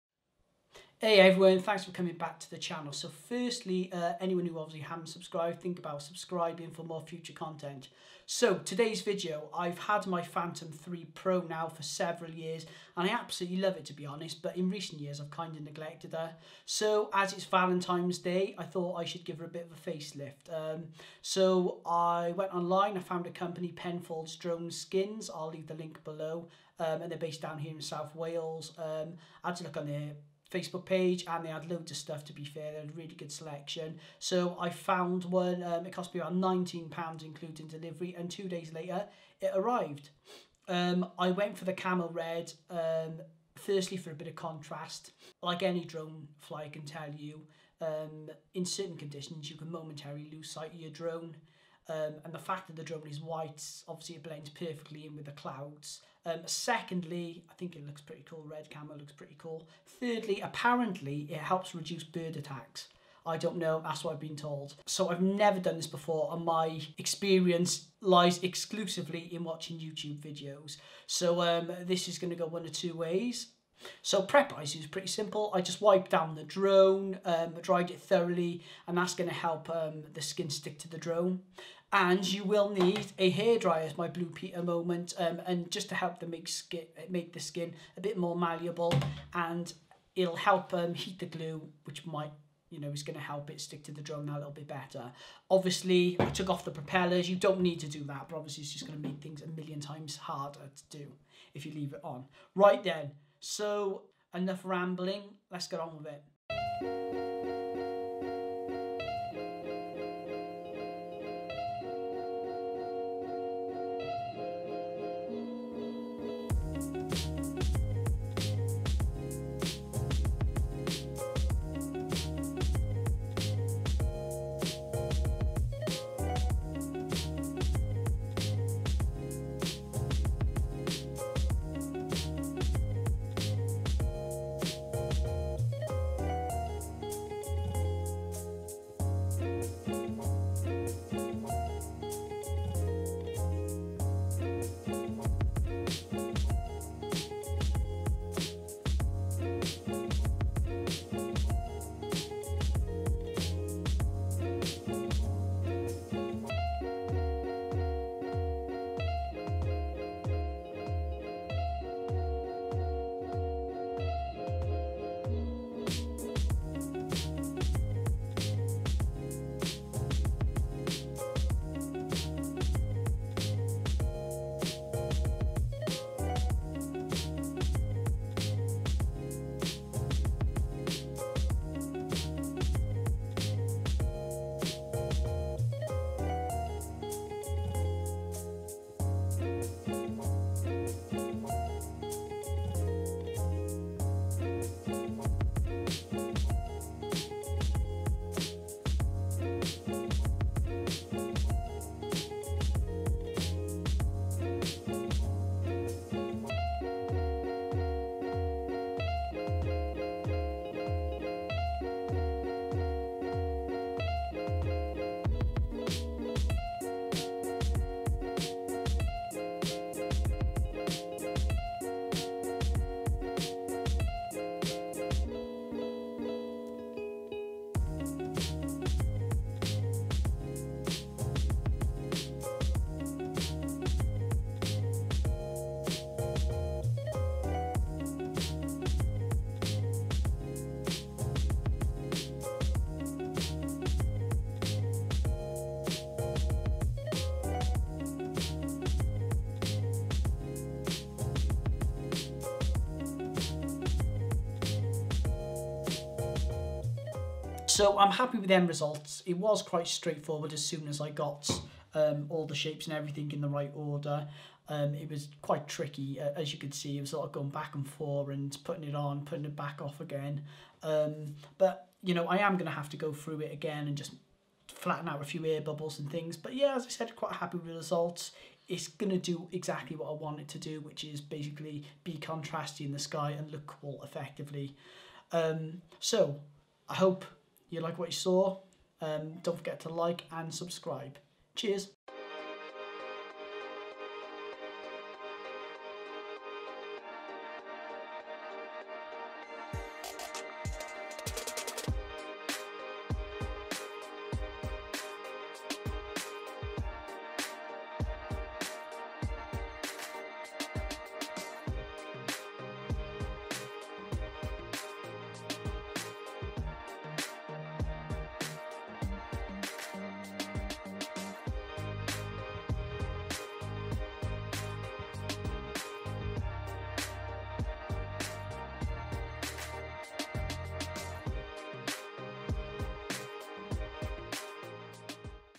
DJI Phantom 3 Pro Body Skin install
So the romantic inside me decided to give my DJI phantom 3 professional a facelift. I found a local company Penfolds Drone Skins down here in south Wales on facebook the Camo Red caught my eye so went with that. So I had a pop and filmed myself doing the modification myself and I must say I impressed myself with the results, im no professional and its anything but perfect but. Body Skin Kits are the future.

Music Used